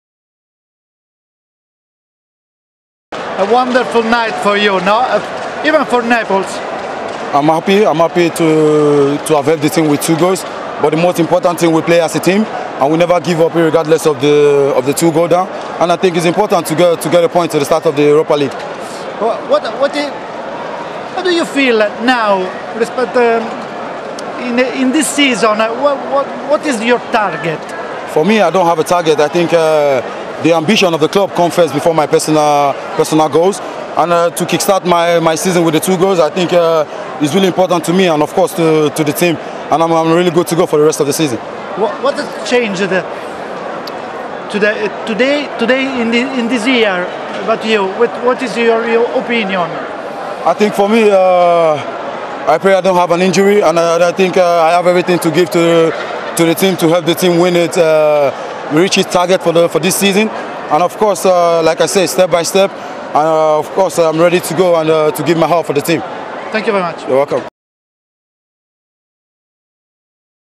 Napoli hero Victor Osimhen on his two brilliant goals vs Leicester.
"To kick start my season with two goals is really important. I'm good to go for the rest of the season."

22-year-old Victor Osimhen rescued a point for Napoli with two brilliant second half goals vs Leicester tonight. He looks like he has a big future!

Watch the UEFA Champions League, Europa League & Europa Conference League live on Virgin Media Television